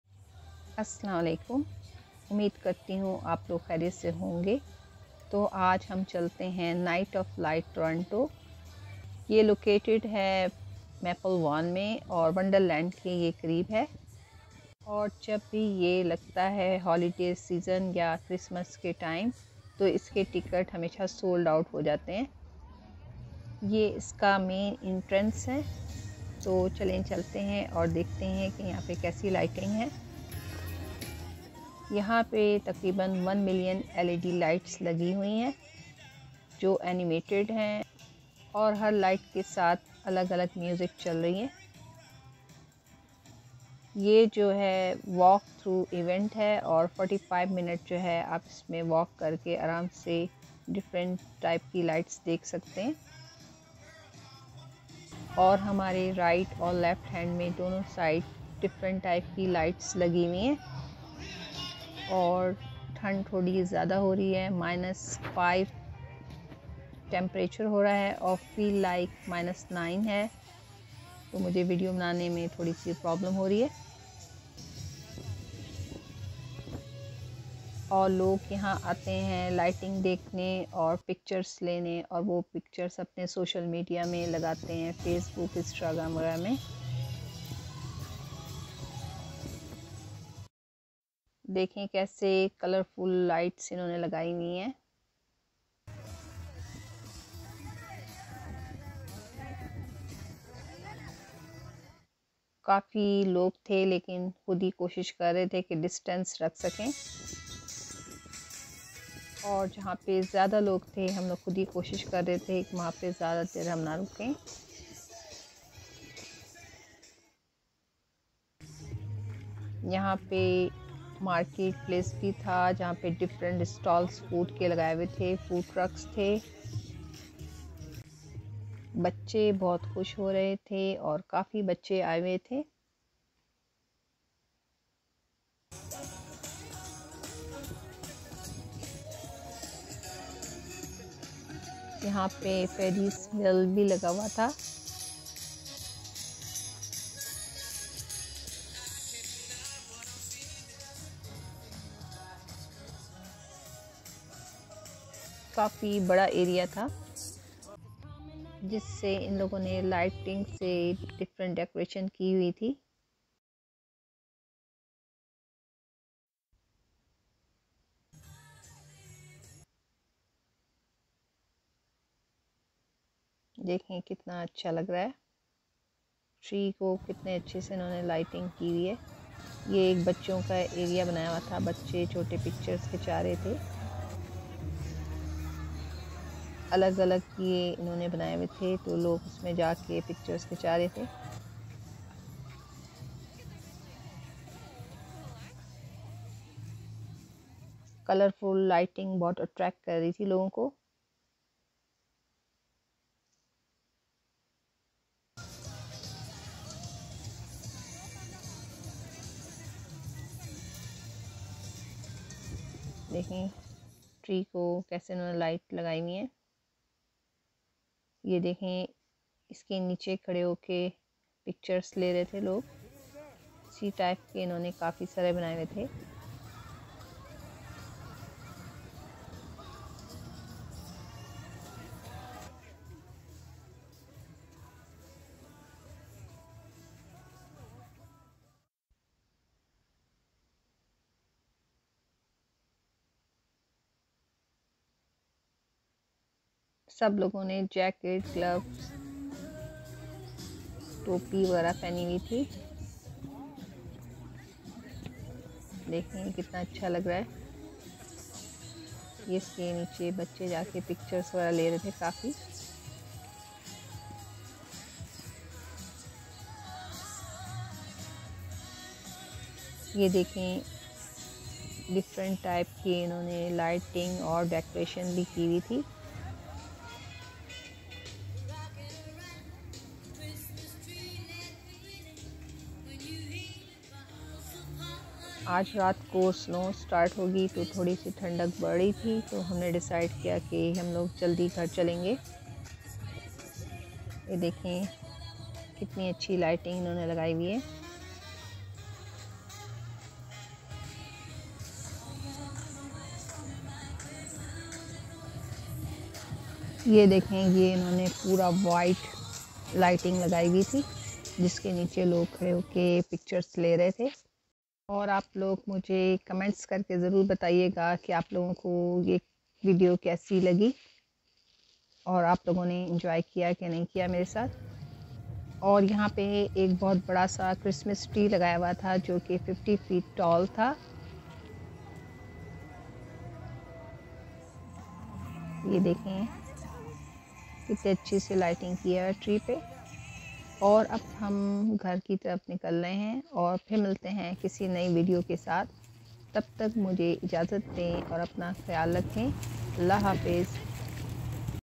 Holiday Vlog | Come to Nights of Lights with me *Urdu/Hindi*
Come to Holiday and Christmas Fair with me, called Nights of Lights located in Toronto, Ontario! Christmas lights and decorations in Toronto, more specifically in Vaughan, Ontario, Canada.